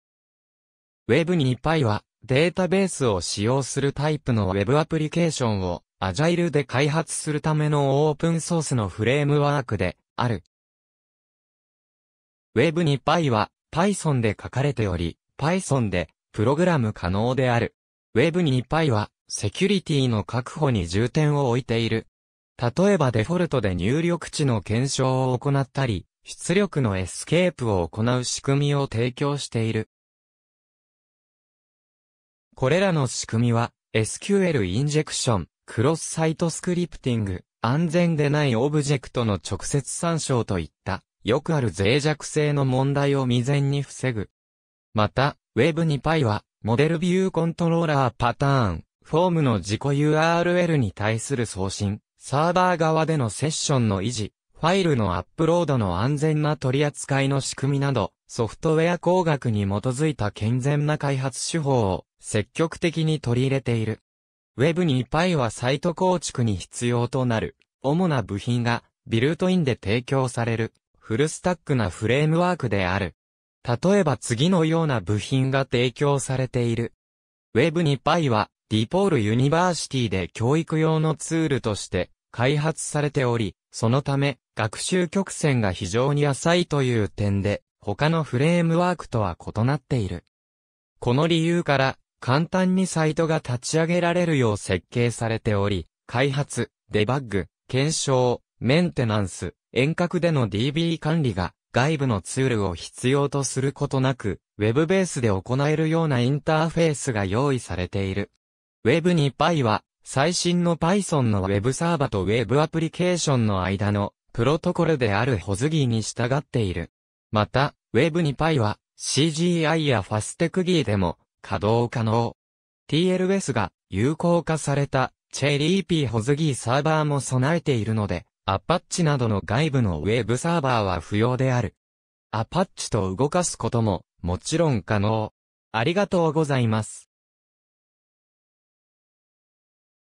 Web2py
web2pyは、データベースを使用するタイプのWebアプリケーションを、アジャイルで開発するためのオープンソースのフレームワークである。
web2py はPythonで書かれており、Pythonでプログラム可能である。
web2pyはセキュリティーの確保に重点を置いている。
例えばデフォルトで入力値の検証を行ったり、出力のエスケープを行う仕組みを提供している。
これらの仕組みはSQLインジェクション、クロスサイトスクリプティング、安全でないオブジェクトの直接参照といったよくある脆弱性の問題を未然に防ぐ。
またweb2pyはModel View Controllerパターン、フォームの自己URLに対する送信、サーバー側でのセッションの維持、ファイルのアップロードの安全な取り扱いの仕組みなど、ソフトウェア工学に基づいた健全な開発手法を積極的に取り入れている。
web2pyはサイト構築に必要となる主な部品がビルトインで提供される、フルスタックなフレームワークである。
例えば次のような部品が提供されている。
web2pyはDePaul Universityで教育用のツールとして開発されており、そのため学習曲線が非常に浅いという点で他のフレームワークとは異なっている。
この理由から、簡単にサイトが立ち上げられるよう設計されており（インストール作業が不要）、開発、デバッグ、検証、メンテナンス、遠隔でのDB管理が外部のツールを必要とすることなく、ウェブベースで行えるようなインターフェースが用意されている。
web2pyは最新のPythonのウェブサーバとウェブアプリケーションの間のプロトコルであるWSGIに従っている。
またweb2pyはCGIやFastCGIでも稼動可能。
TLSが有効化されたCherryPy WSGIサーバーも備えているので、Apacheなどの外部のウェブサーバーは不要である。
Apacheと動かすことももちろん可能。